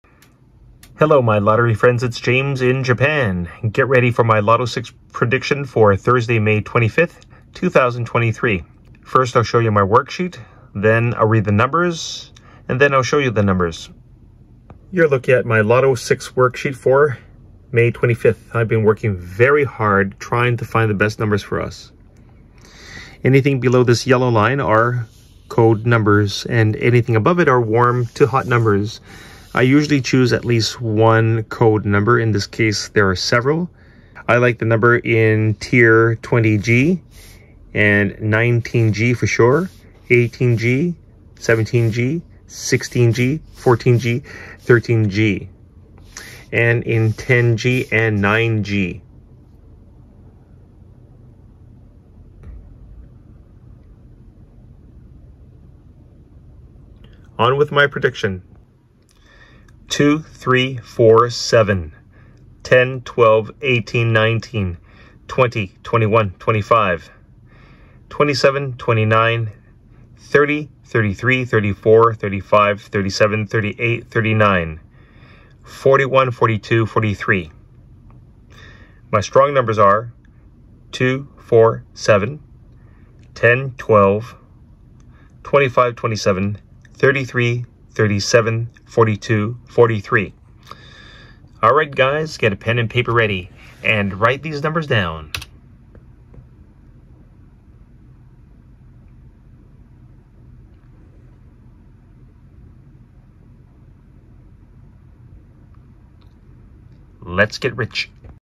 My LOTO6 prediction for Thursday May 25,2023-My prediction captured 4/6 of the main numbers!
Update as of 12:30pm
I am adding numbers 8 and 9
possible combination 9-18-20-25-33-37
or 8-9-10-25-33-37